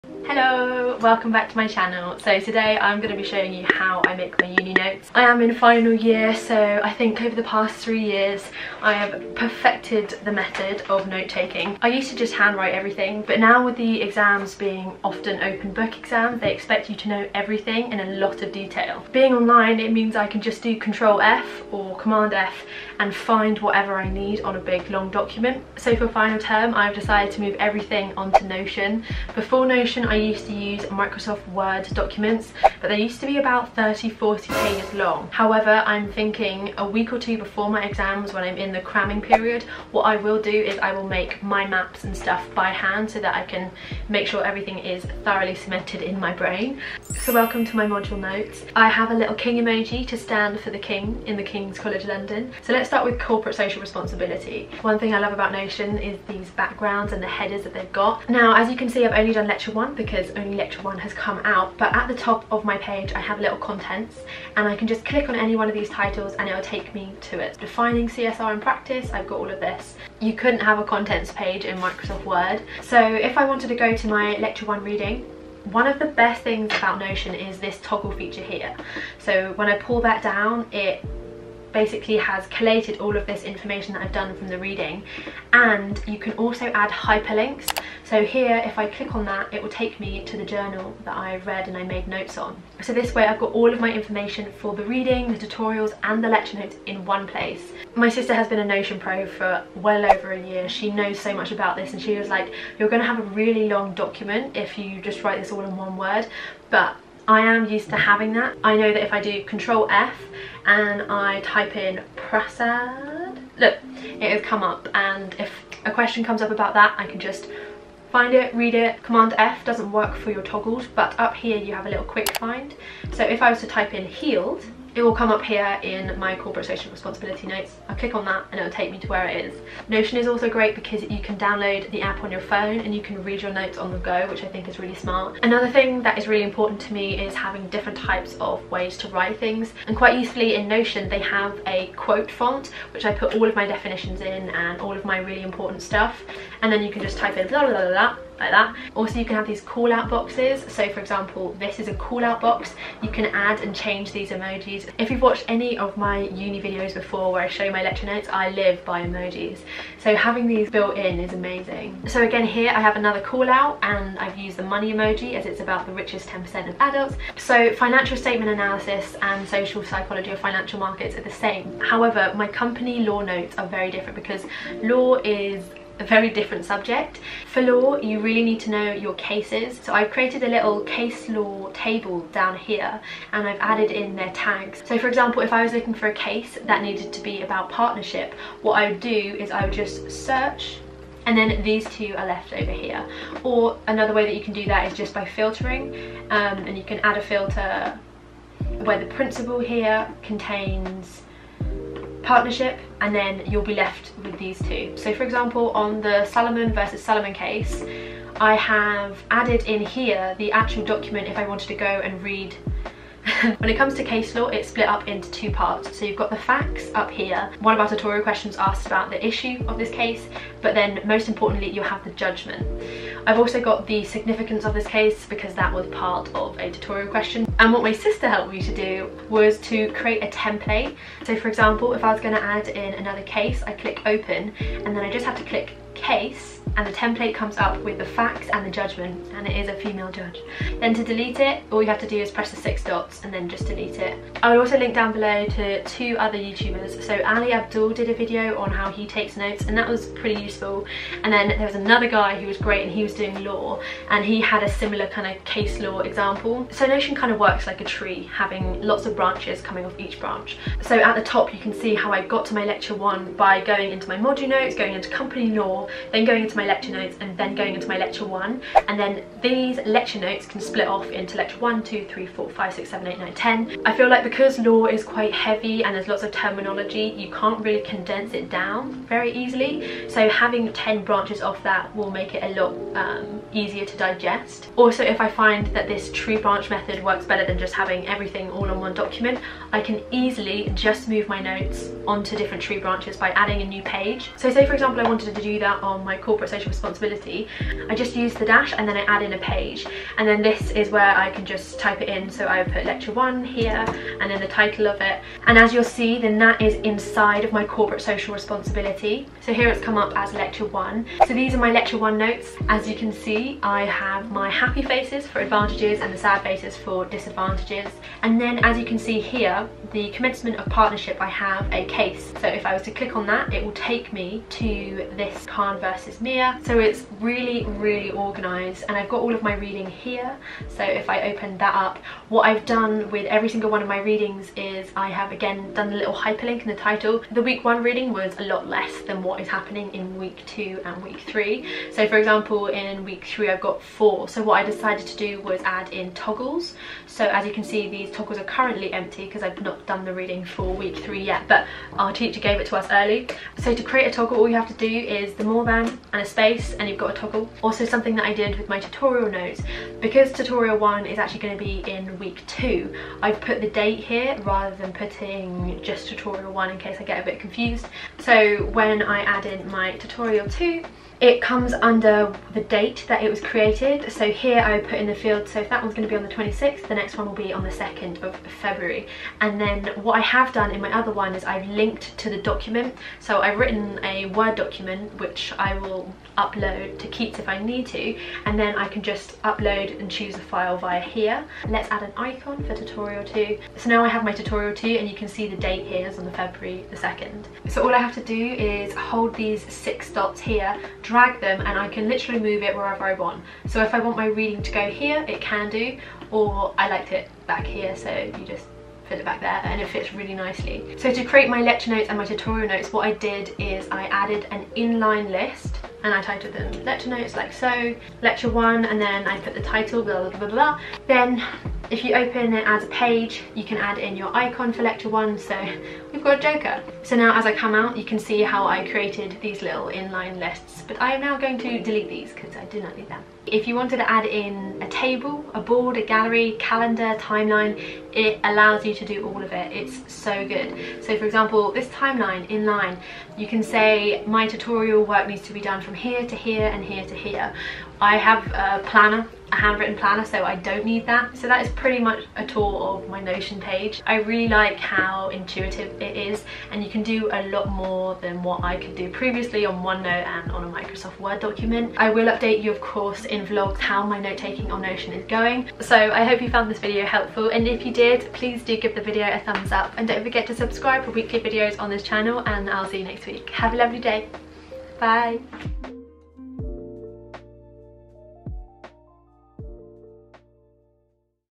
how to take university notes on laptop || my Notion templates
♡ how to take uni notes on notion || university revision ♡

University Note Taking Tips 2021: this is how to make the best first class lecture notes! I never thought I’d be converted to online notes, but here we are!! Enjoy 💜

00:00 intro
00:46 module notes homepage
00:58 lecture notes - contents page
01:12 reading notes - hyperlinks
01:48 command f & control p
02:51 company law notes - the good part of the video
03:33 case templates
04:28 relational databases
05:11 emojis
05:37 reading notes - toggles
06:50 inline lists
07:13 calendars & tables
07:41 goodbyes

Discounts!

University Advice 2021.  If you're looking to find a way to make the best first class lecture notes ever then this video is for you.